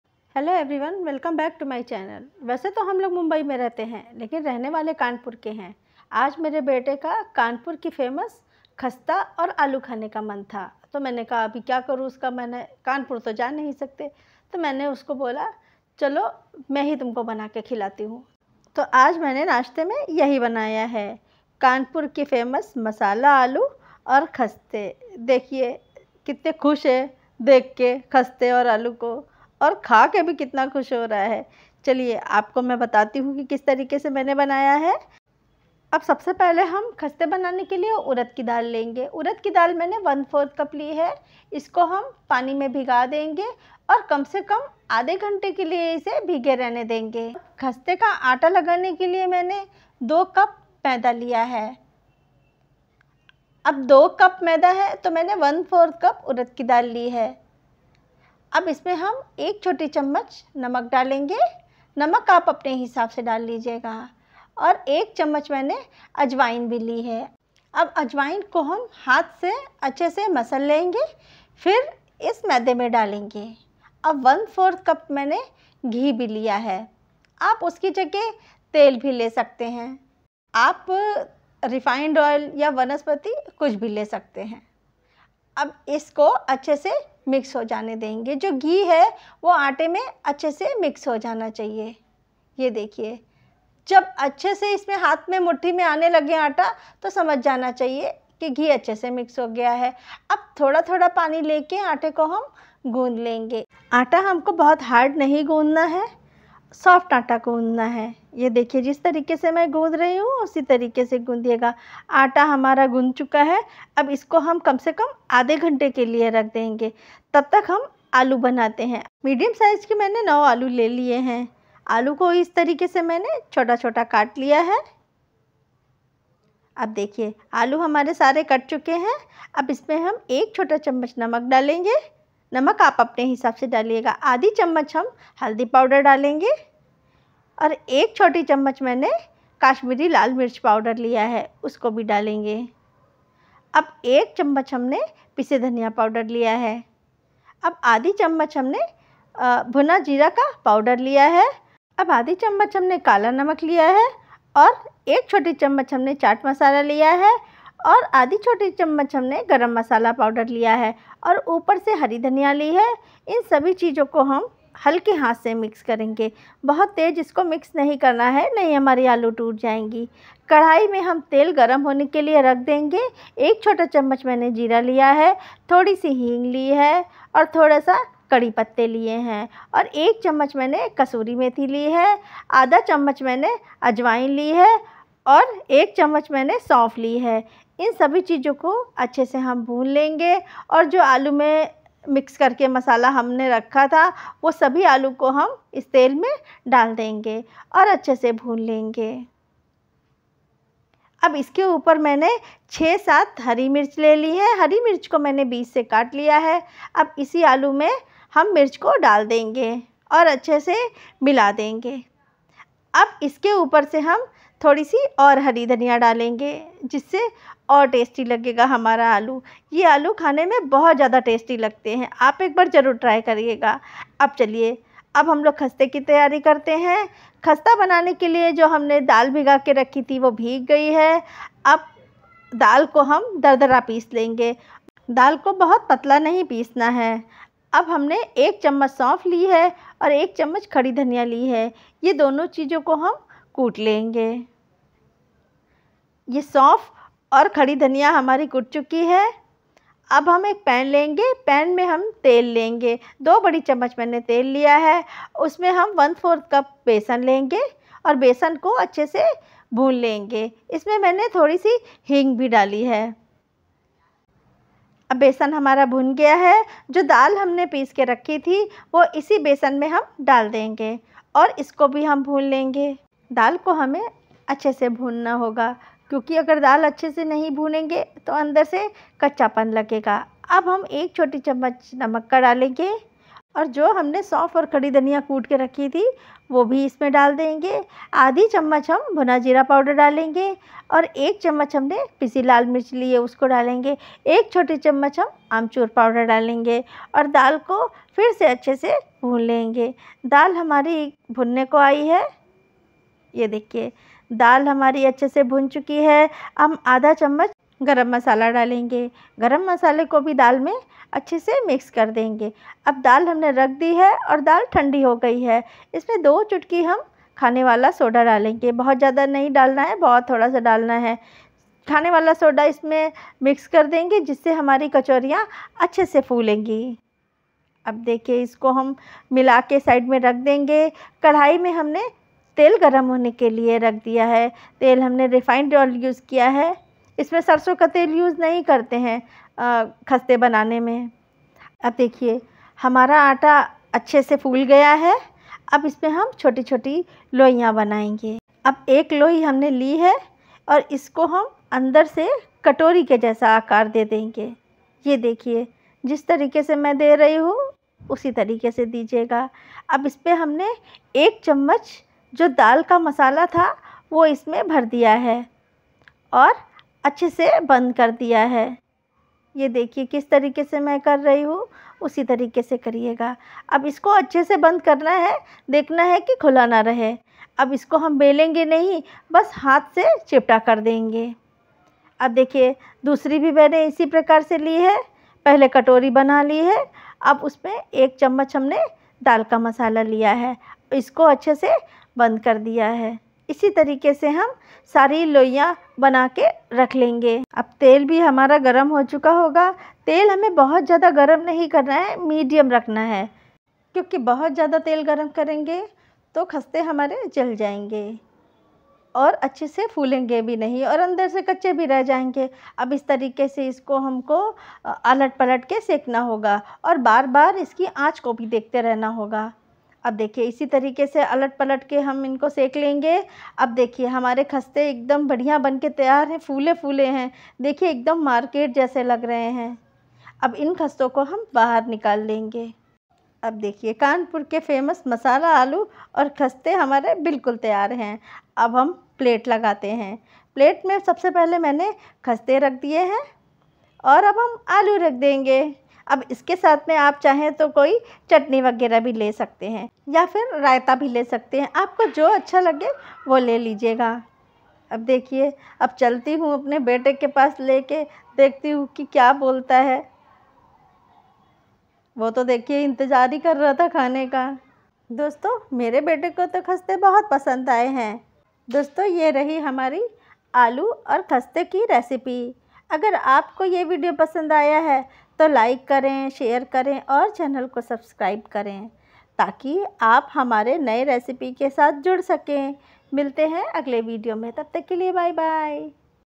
आज मैंने नाश्ते में बनाया है कानपुर का मशहूर खस्ता और मसाला आलू की सब्जी | Khasta Kachori Recipe
Is video mein maine Kanpur ki famous khasta  Kachori aur aloo ki sabji banane ki recipe batayi hai.